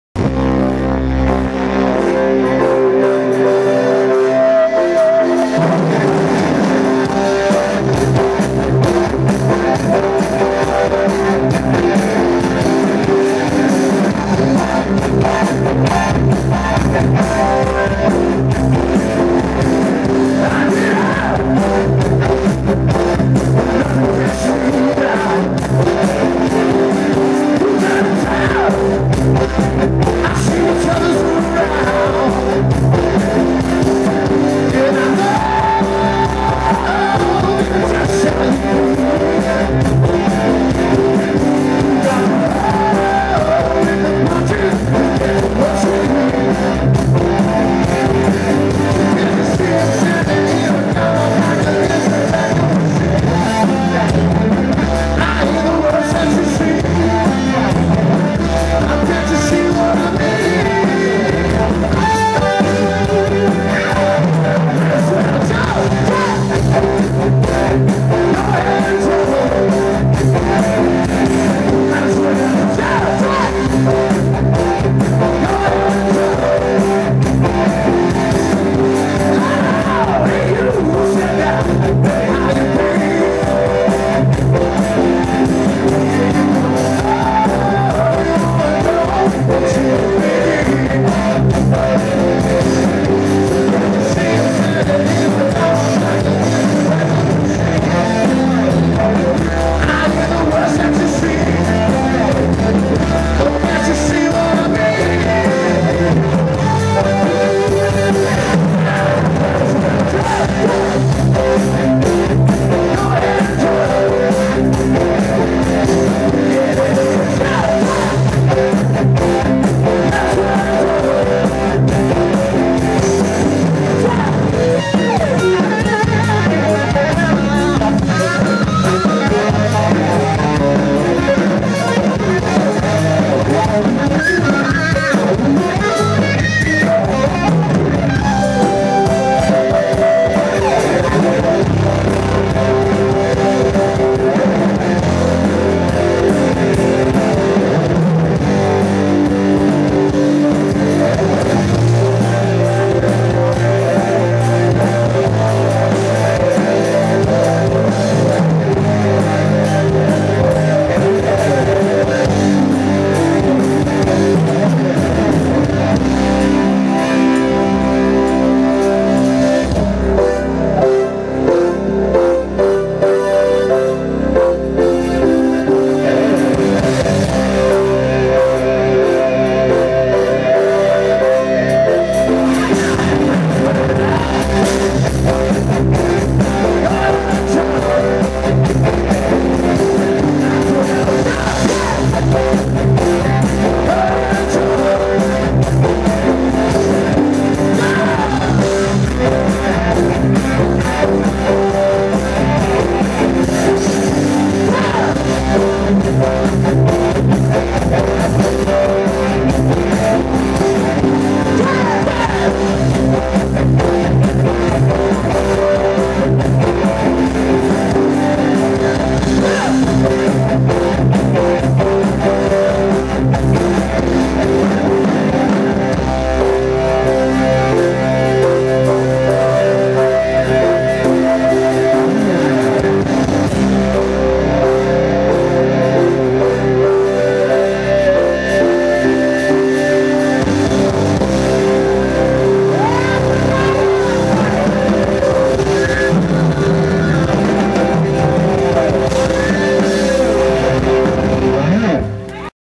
Rotaryfest 2012 Mustang Heart at The Stage Sault Ste. Marie
Mustang Heart play at The Stage Sault Ste. Marie